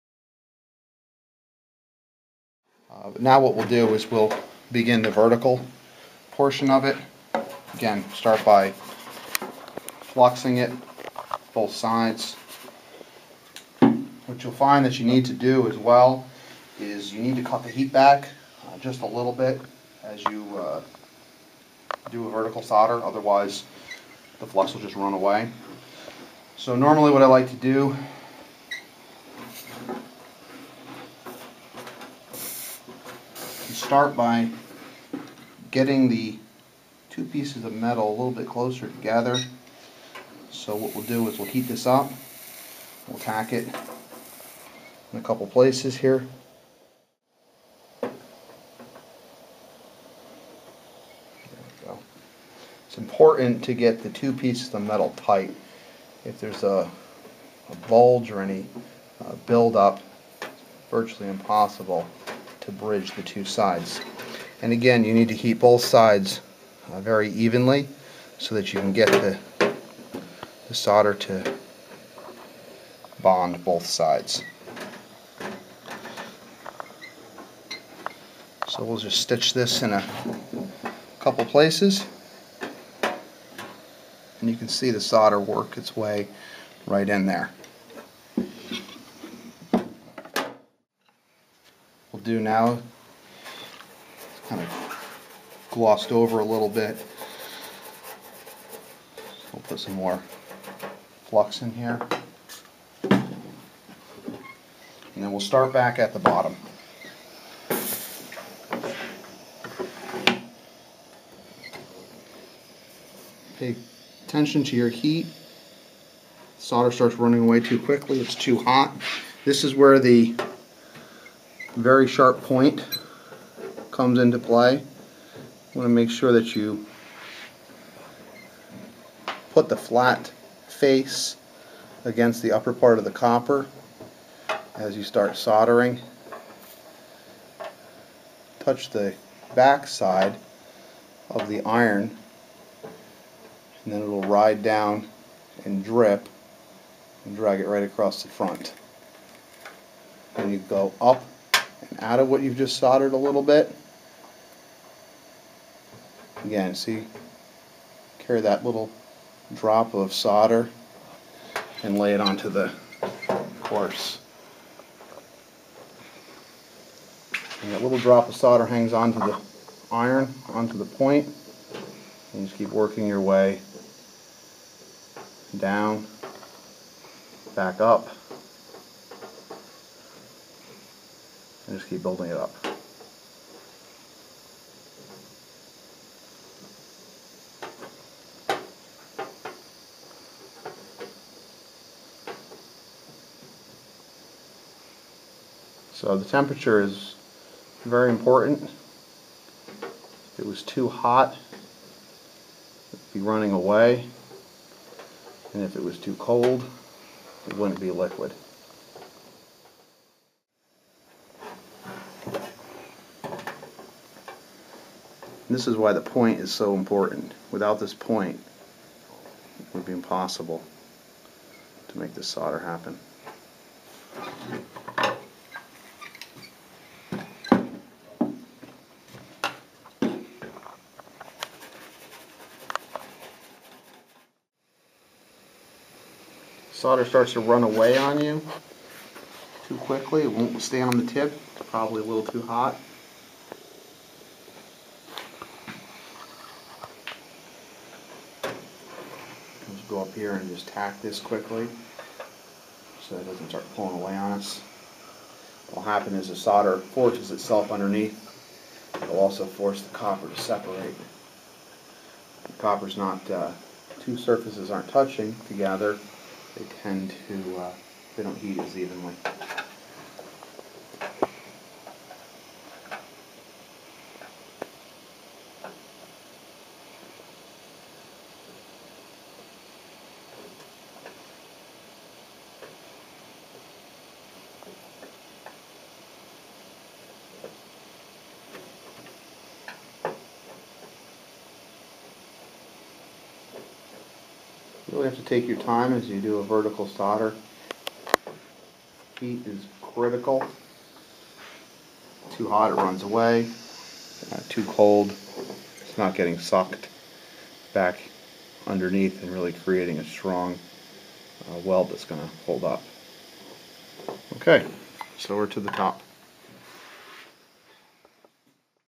Part 4: Soldering a Vertical Seam | New Dimension Construction
Kyle Diamond of New Dimension Construction demonstrates how to solder a vertical seam, the 4th step in a 5 part series.  This video series was done in conjunction with JLC for an article featuring a built-in gutter rebuild with a copper lining.  The gutter project will also featured on New Dimension Construction's blog.